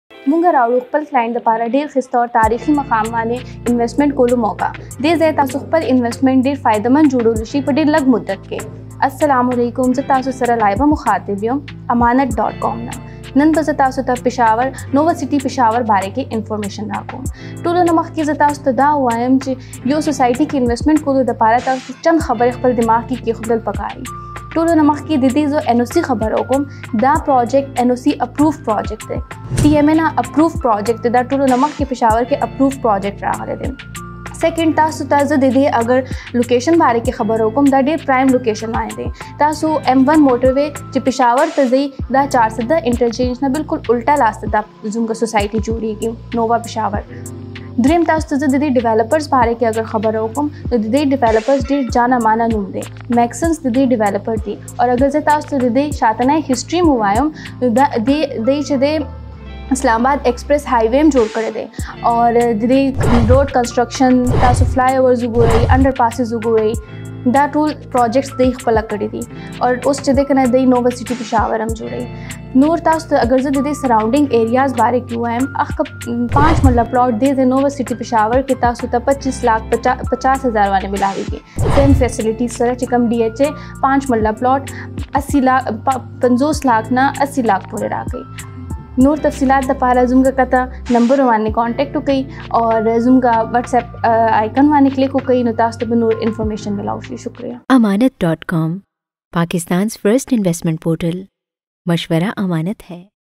Nova City Peshawar General Overview | Latest Updates of Nova City Peshawar | Ammanat
Nova City Peshawar is a project of Nova developers. Nova developers are not new name in real estate business. Therefore, people trust and invest in the Nova group projects for their reliability. Moreover, Nova developers are appreciated both nationally and internally. Nova City has been officially launched in the end of January 2022. In fact, it has already received tremendous response from the developers. Nova developers are a team of highly skilled people. Additionally, Nova Peshawar is developed based on the state of the art infrastructure and technology.